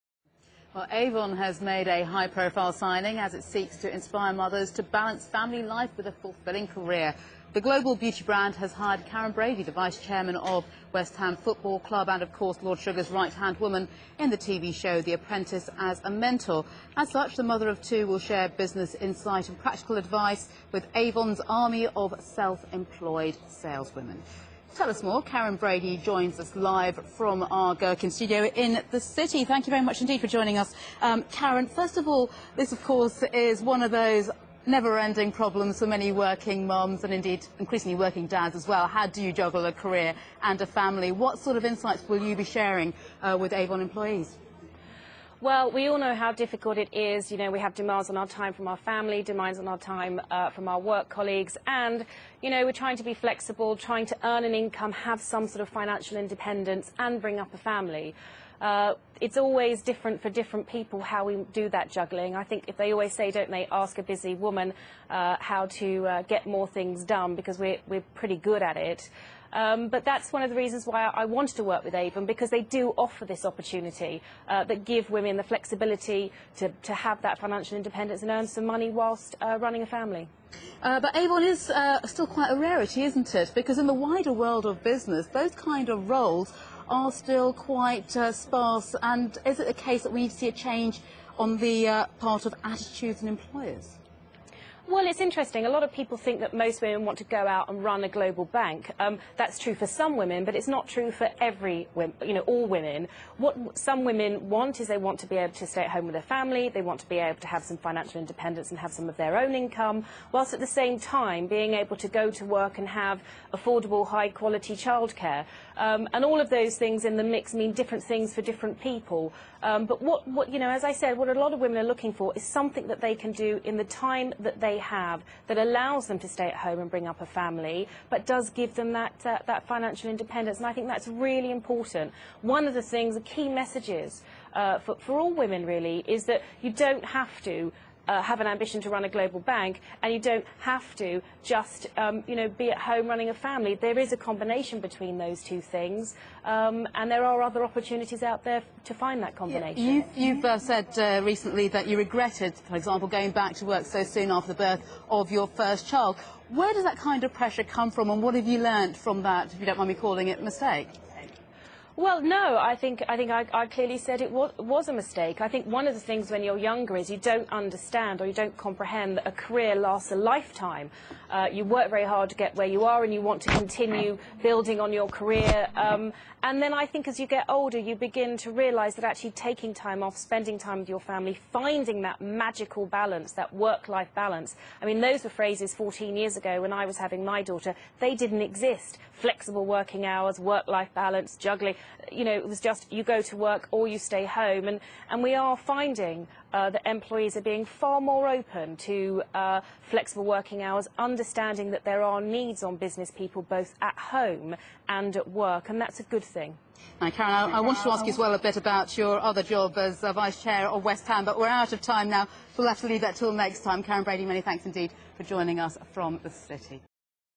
Karen Brady on Avon Cosmetics
Karen Brady promotes Avon Cosmetics as you can balance family life with a career.